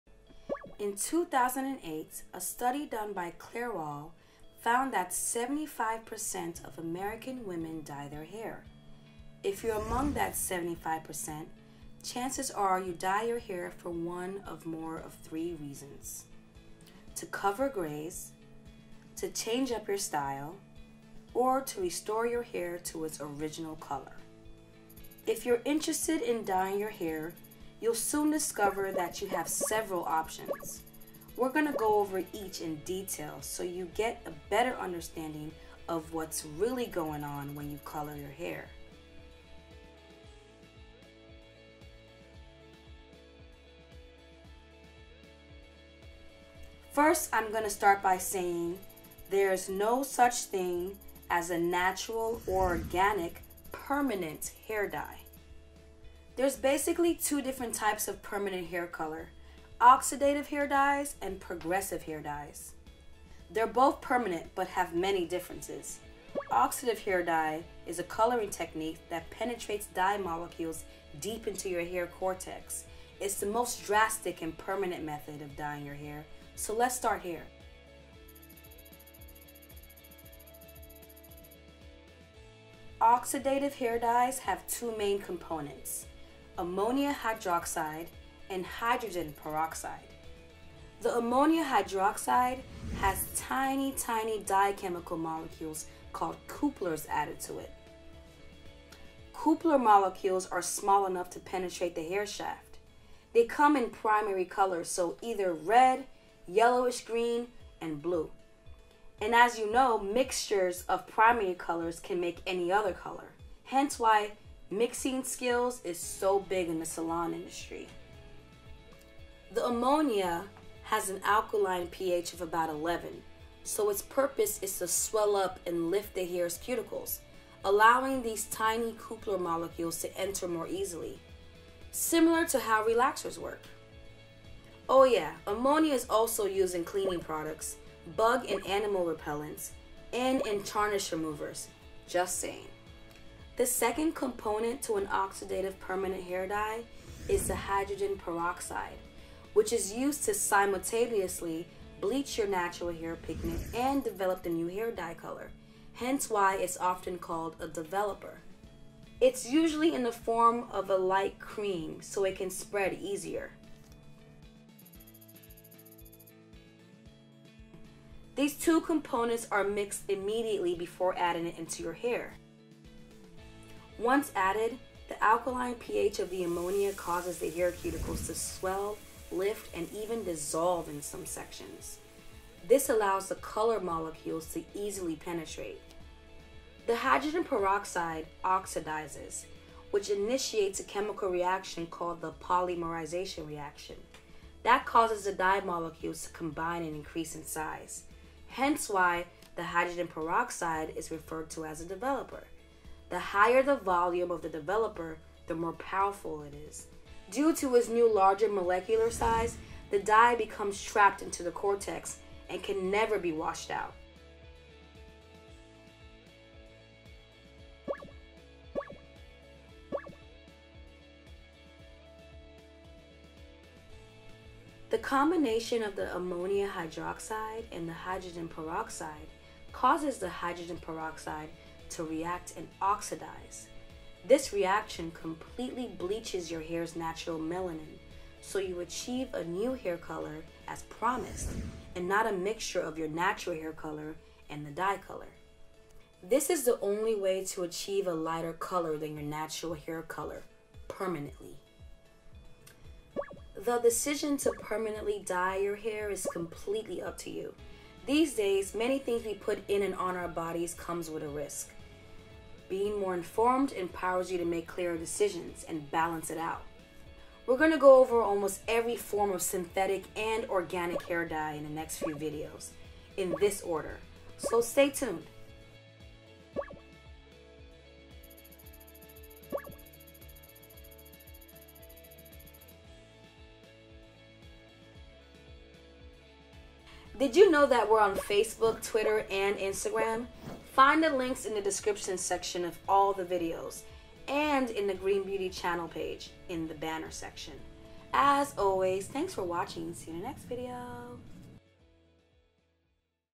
What happens when you use Permanent Oxidative Hair Dye
Oxidative hair dye is the most drastic and permanent hair dye method, so it’s often used by people who want to go blond or get more unusual colors like blue or pink.